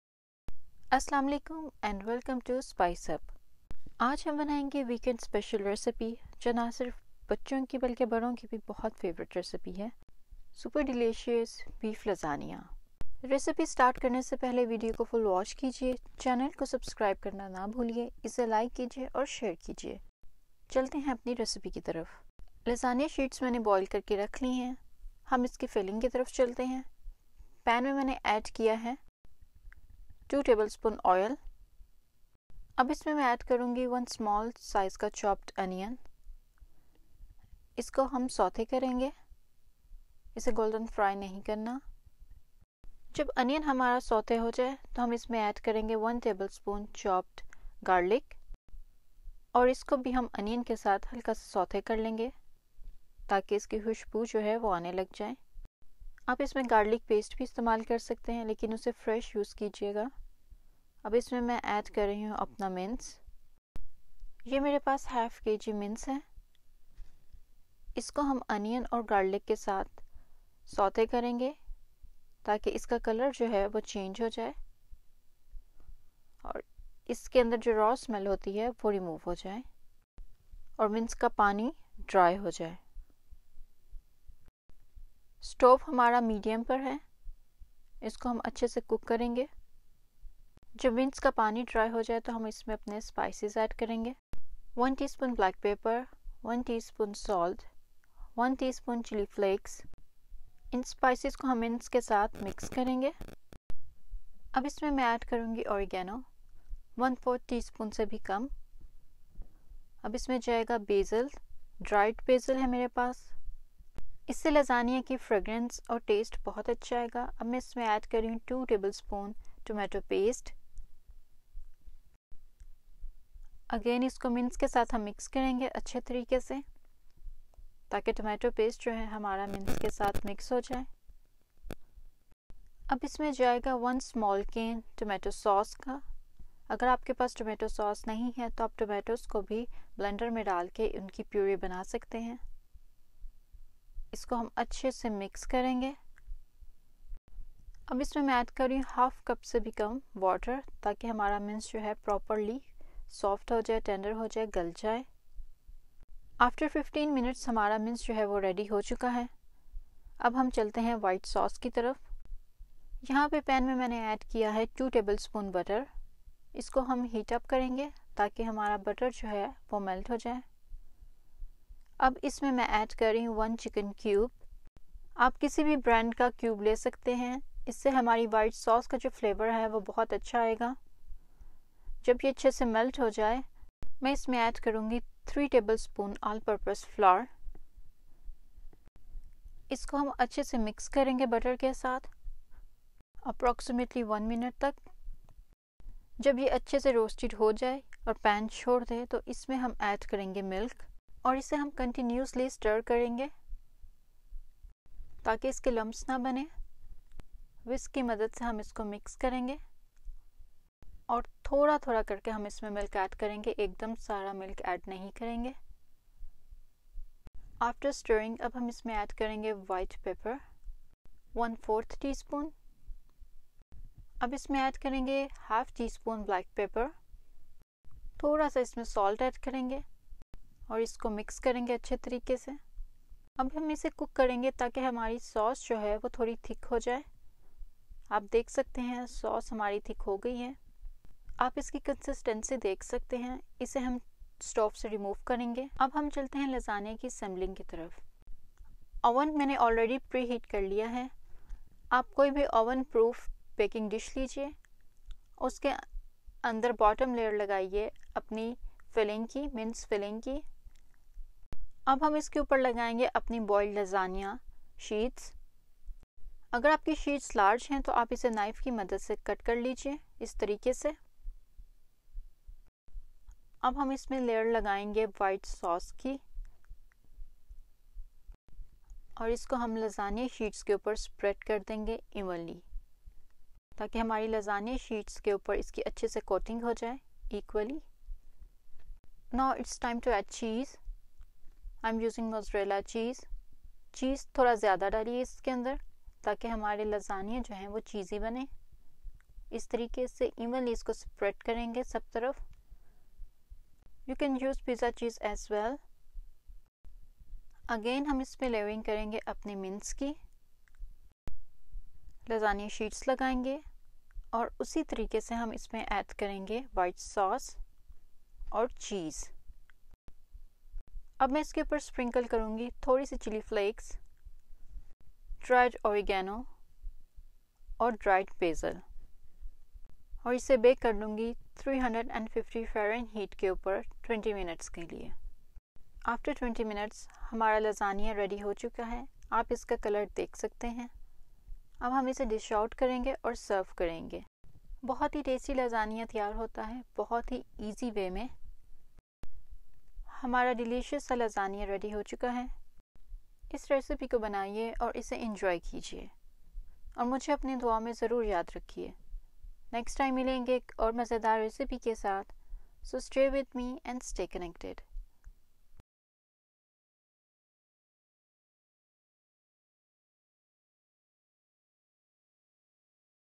Beef lasagna||easy and perfect recipe ||cheesiest,Easiest||Subscribe,share,like
Welcome to my Spice up Family in another yummy recipe.today’s recipe is most demanding recipe,everyone’s enjoy it a lot.Beef Lasagna.